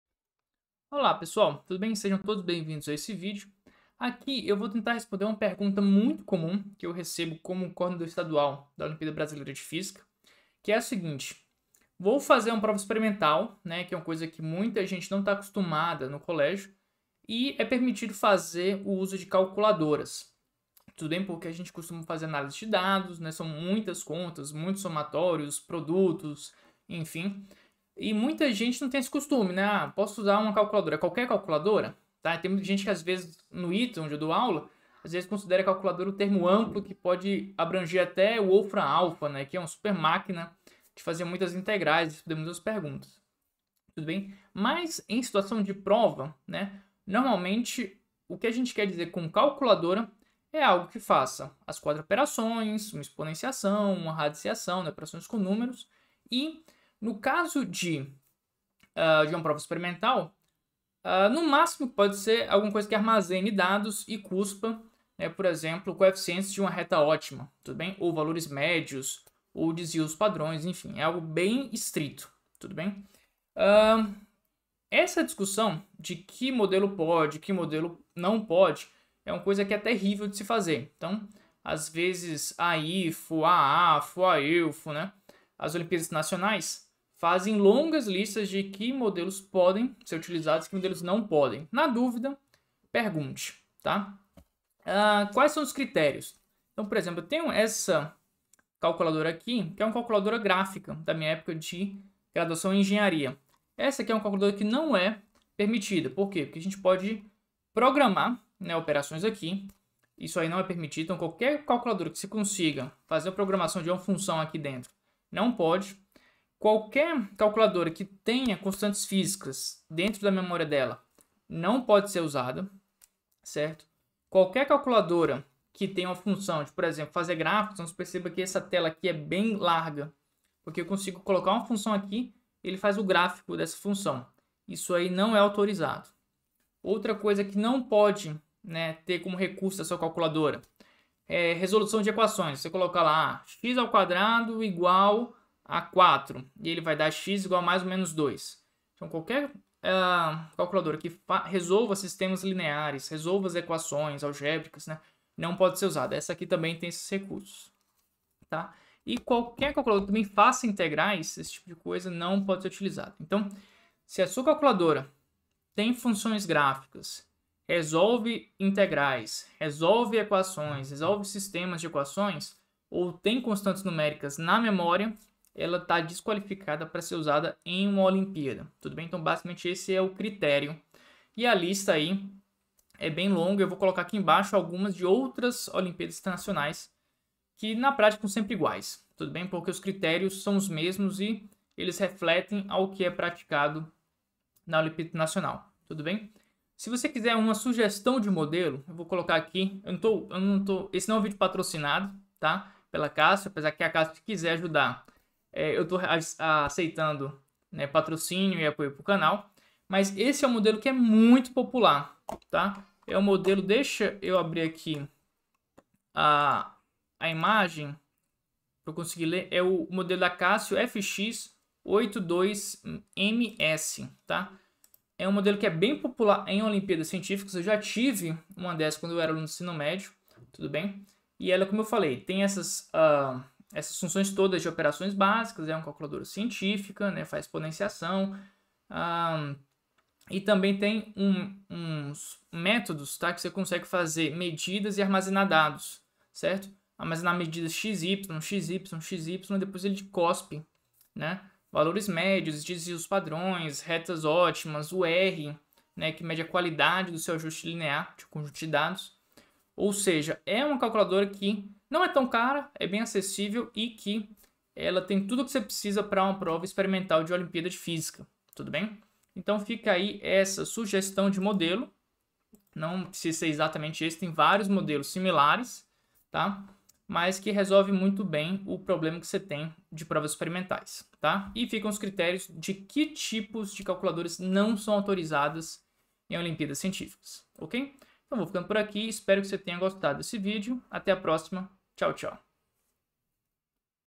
Calculadoras permitidas em Olimpíadas Científicas
Aprenda física experimental e a usar funções avançadas de calculadoras científicas

Que tipo de calculadora pode ou não ser usada em uma olimpíada científica? Respondo a pergunta usando o critério das olimpíadas de física.

A ideia desse vídeo é ajudar os estudantes com as práticas comumente adotadas pelas olimpíadas. Em caso de dúvida, consulte o regulamento da competição ou pessoa responsável.